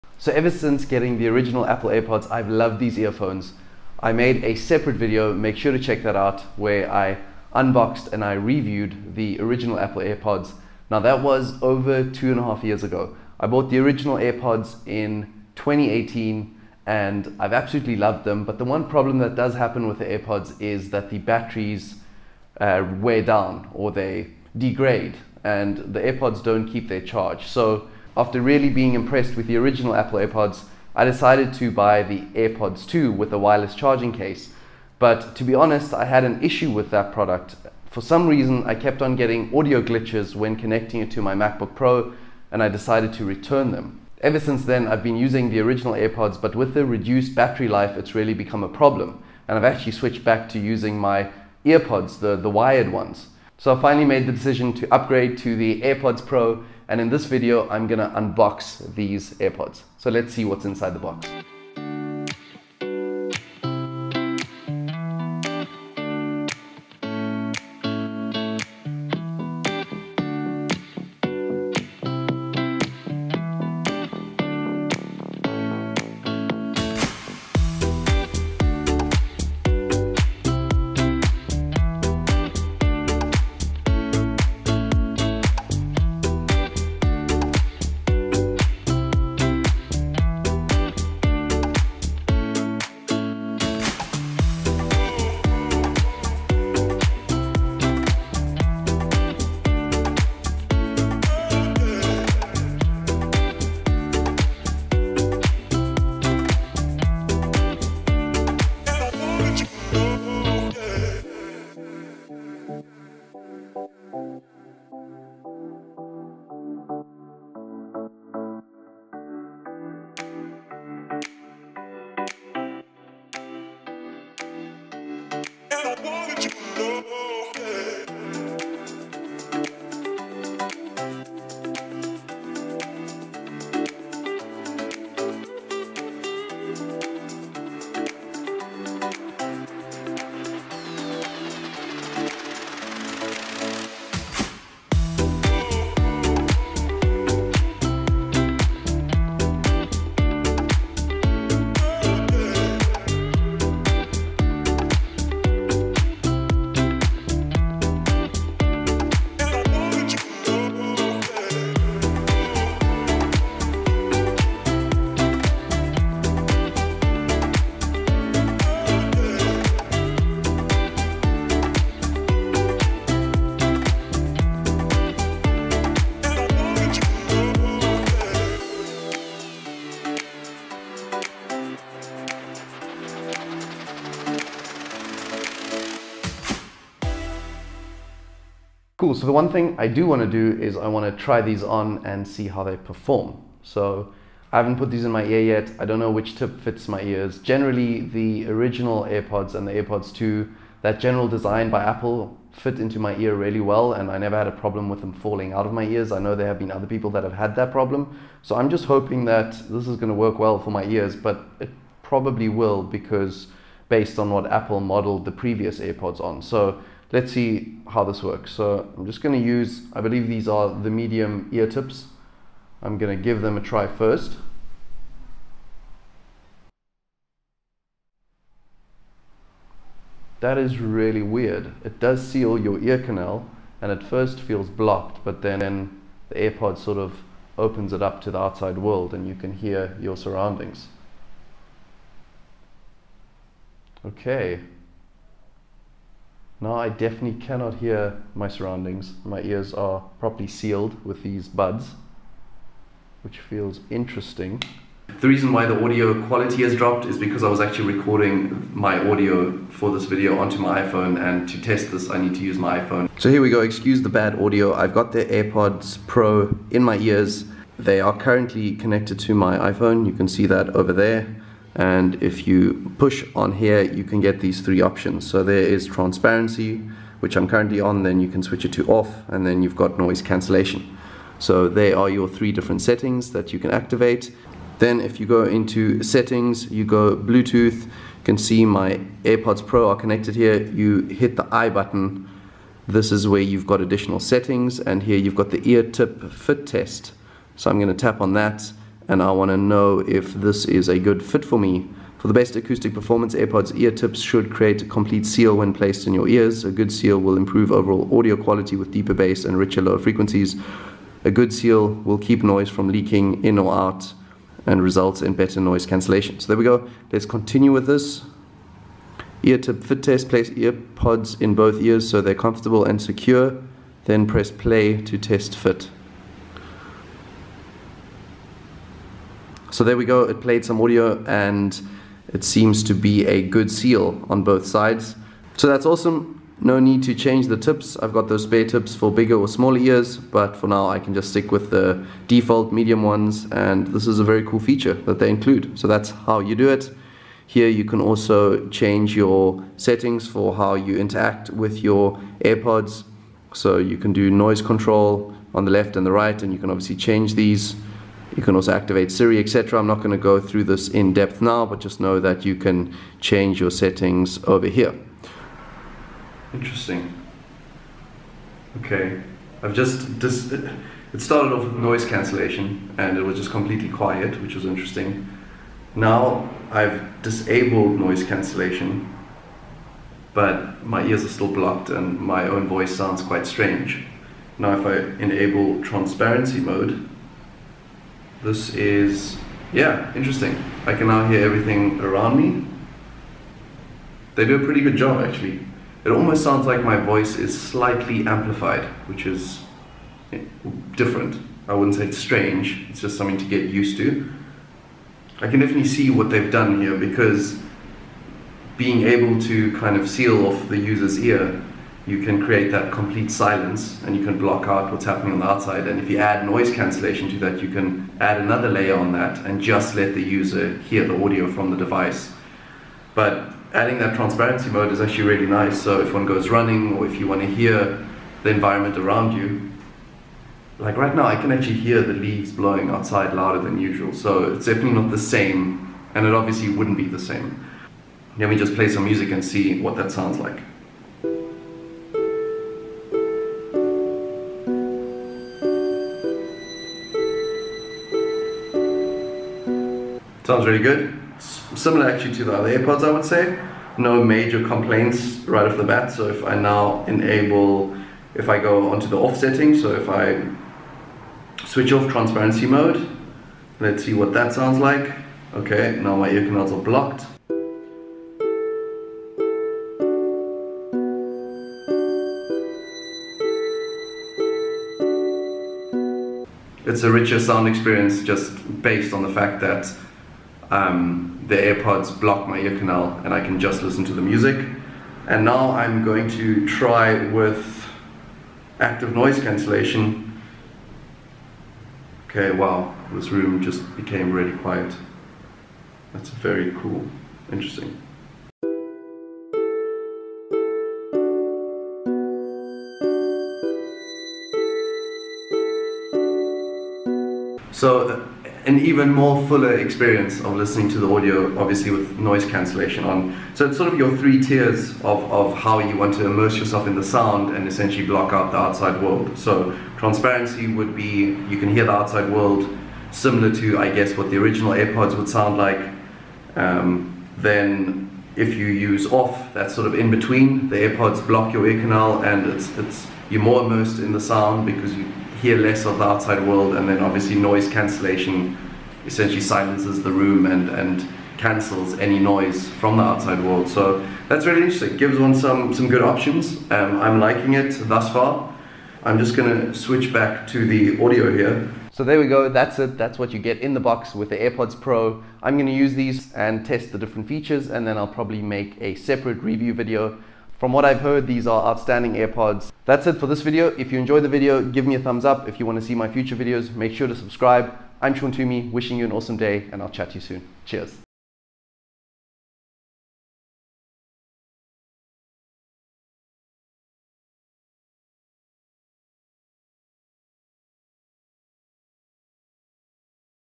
Apple AirPods Pro Unboxing, Setup & Simulated Audio Experience
In this video I unbox the AirPods Pro, walk through the setup process, ear tip fit test and simulate the audio experience for all 3 modes — Transparency, Off & Noise Cancellation.